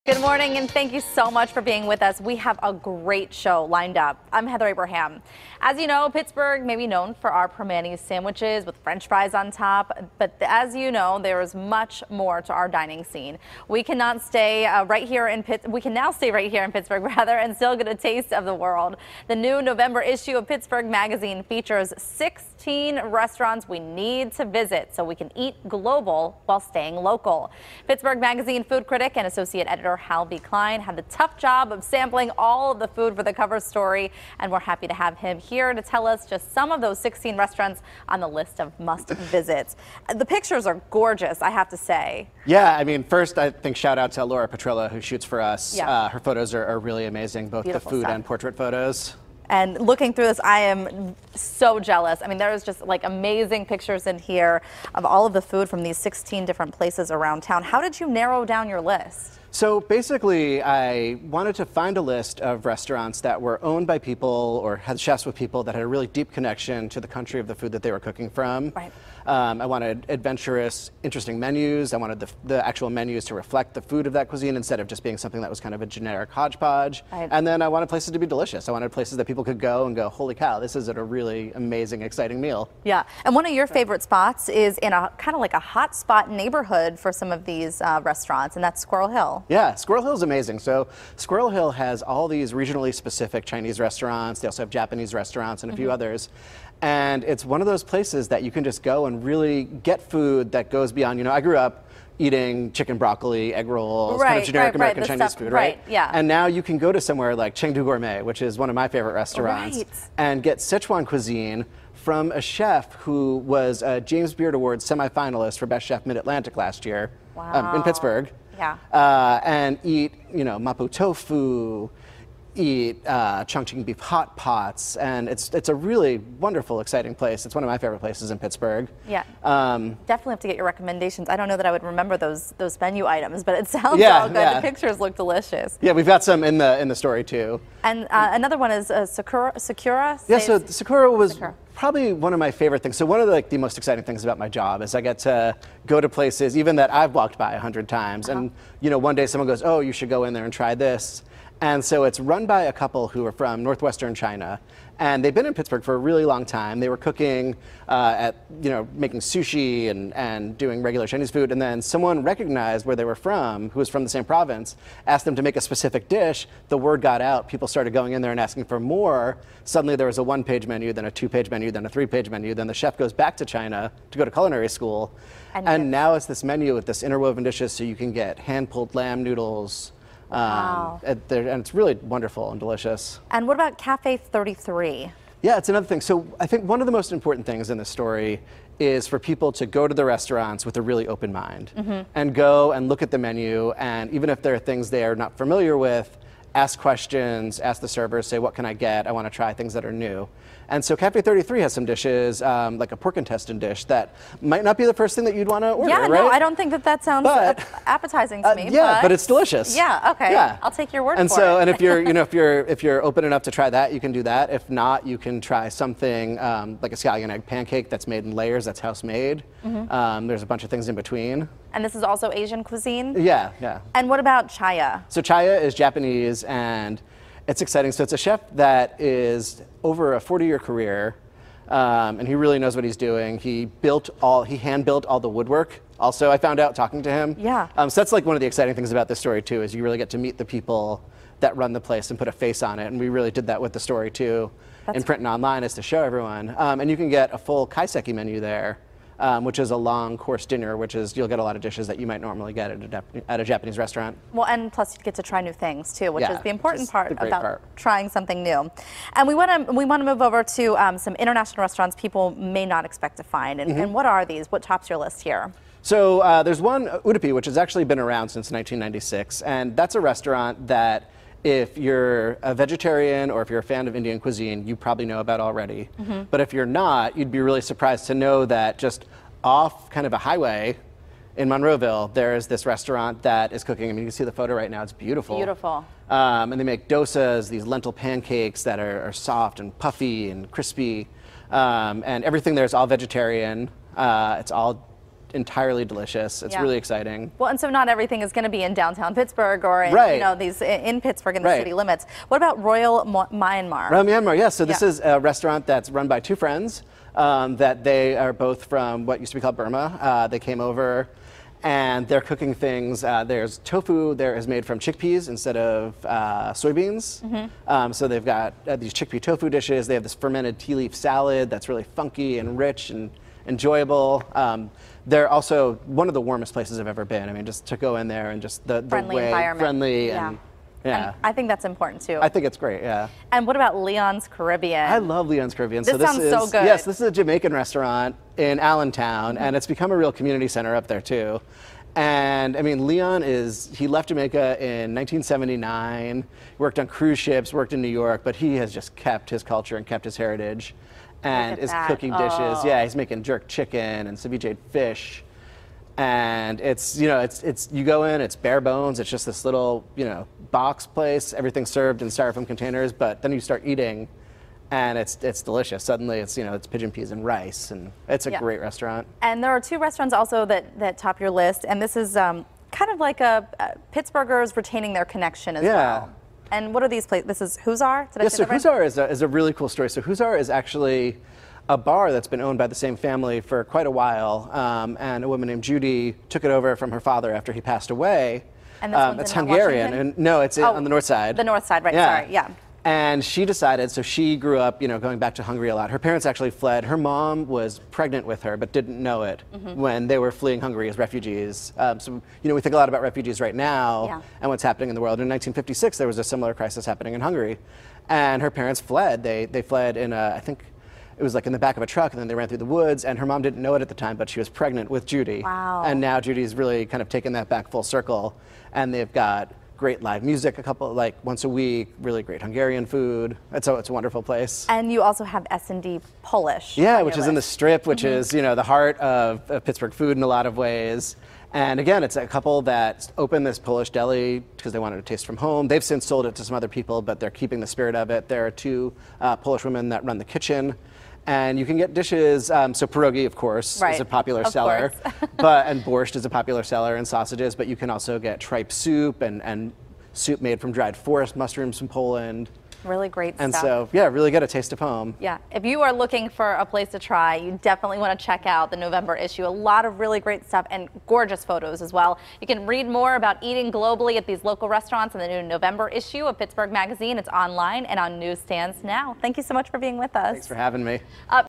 Pittsburgh Magazine’s 16 Must-Visit Restaurants
Hal B. Klein stops by to talk about 16 must-visit restaurants featured in November’s edition of Pittsburgh Magazine!